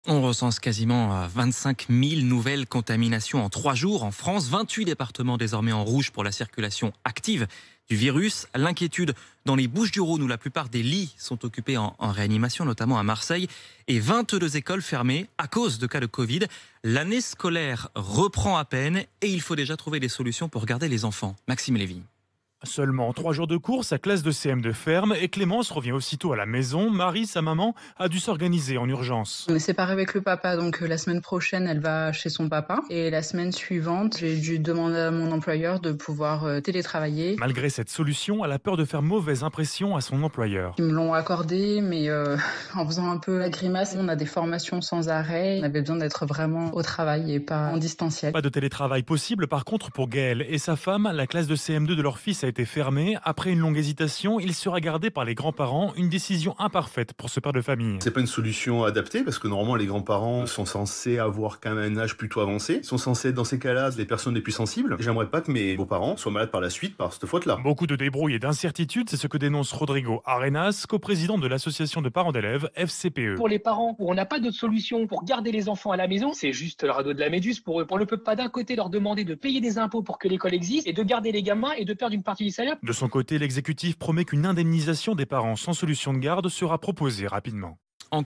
Écoles fermées à cause du Covid: il faut déjà trouver des solutions pour garder les enfants
22 écoles fermées à cause de cas de Covid. L'année scolaire reprend à peine et il faut déjà trouver des solutions pour garder les enfants